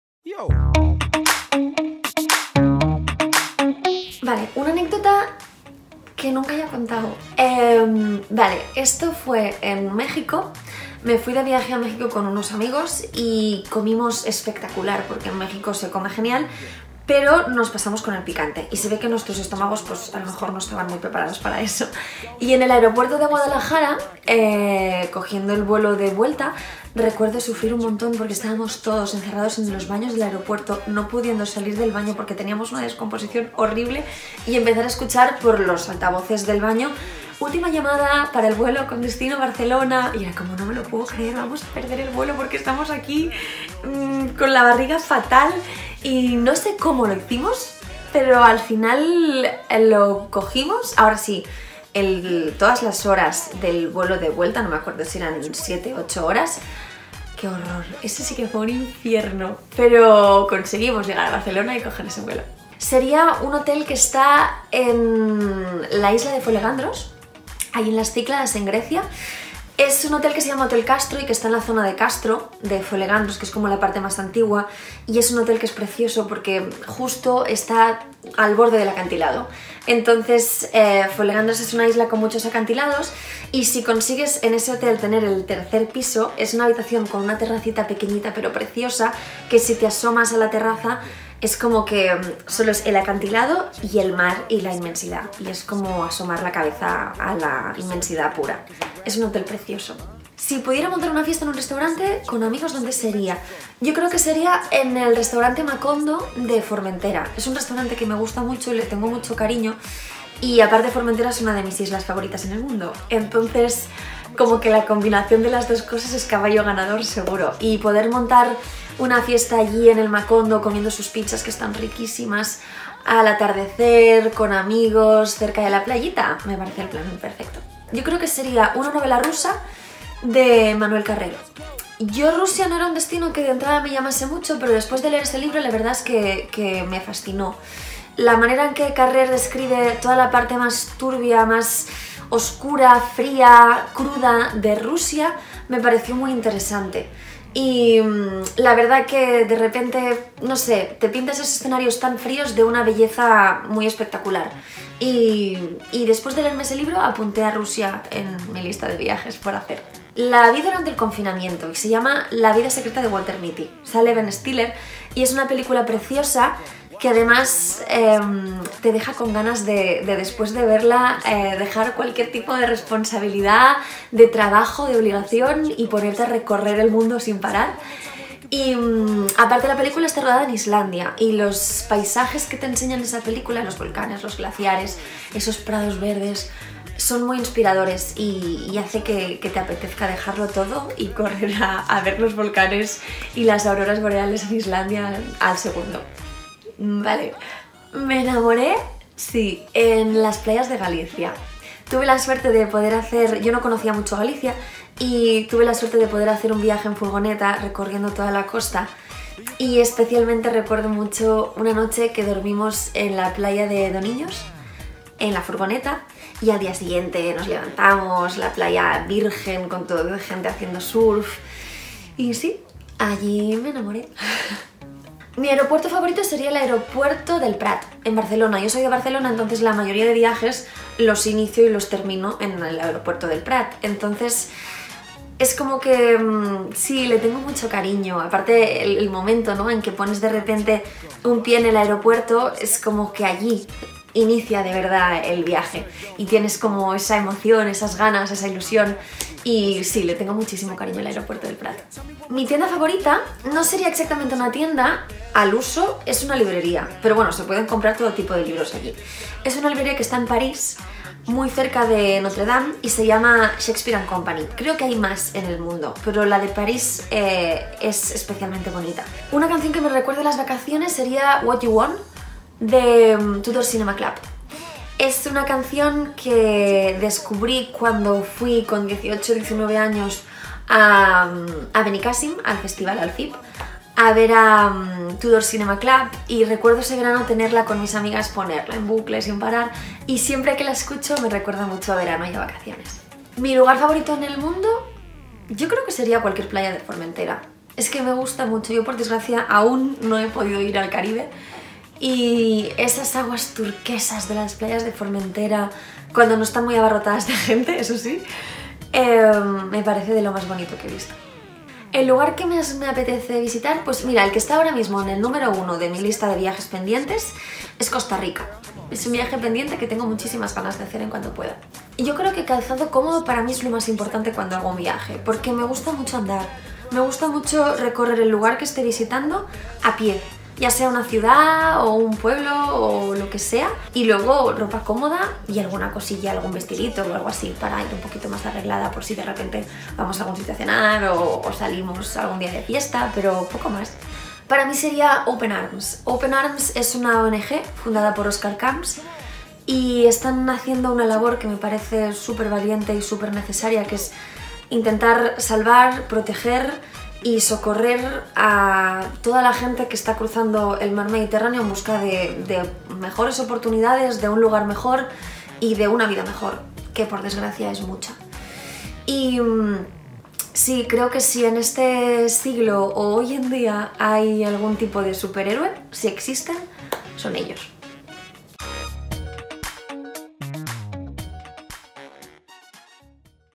YoSoyTraveler Quiz: Paula Malia | CN Traveler España
Paula Malia (Valeria, El Vecino, Loco por ella) ama Formentera por encima de todas las cosas. En la isla fue feliz y a ella quiere volver para disfrutar de las aguas mediterráneas. Pero fue en otra isla (una griega) en la que descubrió el hotel más bello. Paula tiene pendiente viajar a Costa Rica, y a la Rusia de Carrère. Eso sí: siempre con una buena zapatilla en la maleta para caminar y caminar y caminar...